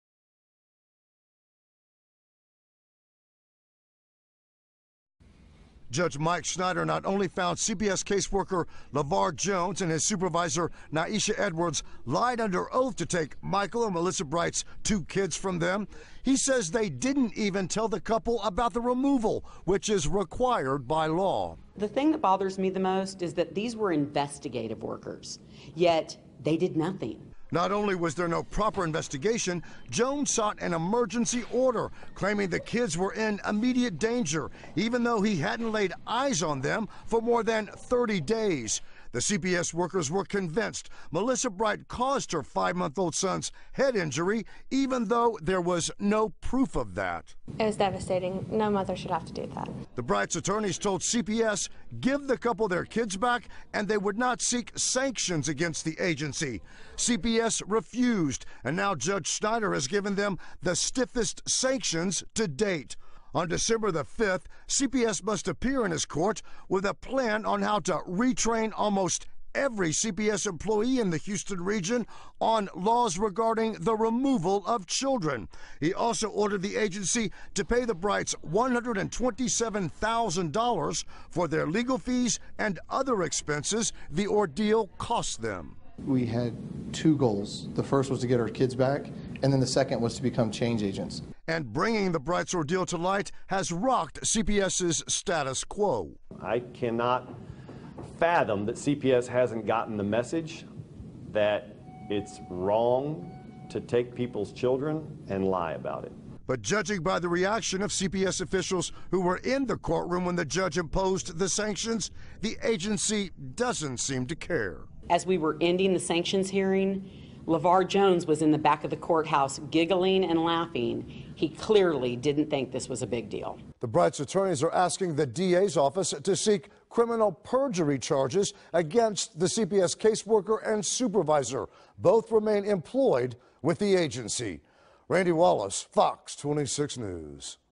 Dad vs CPS - judge orders Largest sanctions against CPS November 9, 2018 -Bright Family Case Video2
Judge orders largest sanctions ever against CPS for lying to remove kids

Bright Family case Video 2

By: Randy Wallace, FOX 26 News

HOUSTON (FOX 26) - "This is by far the largest sanction I've ever been a part of or ever heard of being imposed against CPS," said attorney Dennis Slate.
Family Law Judge Mike Schneider says he would have made Child Protective Services pay even more but didn't want to burden taxpayers.

The judge found CPS case worker Levar Jones and his supervisor Niesha Edwards lied in order to take Michael and Melissa Bright's 2-year-old daughter and 5-month-old son away from them.

"And the fact that CPS still has those two people employed is disgusting," said attorney Stephanie Proffitt.
"I think people would be really concerned if they actually sat here and listened to all of these days worth of CPS covering up the lies they told," Slate said.
The Brights' 2-year-old daughter ended up being abused in foster care.
"We've got a little girl with a black eye and basically an acidic burn in her diaper area," Proffitt said. "She didn't even make it to foster care until 10 or 11 o'clock at night and by the next morning all of these things had already happened to her."

Judge Schneider ordered CPS to pay the Brights $127,000 for legal fees and other expenses for a case he said should have never been brought against them.
"I'm glad it's over. We can just go on and be a family," said Melissa Bright.
"We can go home free and clear without any allegations with our children," Michael Bright said.
Judge Schneider also gave CPS until December 5 to come up with a plan to retrain almost every CPS worker in the Houston region.
"The law on removal, the process on removal, and making sure parents are told about the removal of their children," said Slate.

"It's an unfortunate epidemic and until someone like Judge Schneider makes CPS accountable it's going to continue," Proffitt said.
In a statement CPS says," In light of today's ruling we are reviewing our options including our right to appeal."
But if CPS appeals and loses, the judge said the agency will have to pay all legal expenses.

Randy Wallace fox 26 news reports.

Credit to Randy Wallace reporter and Fox26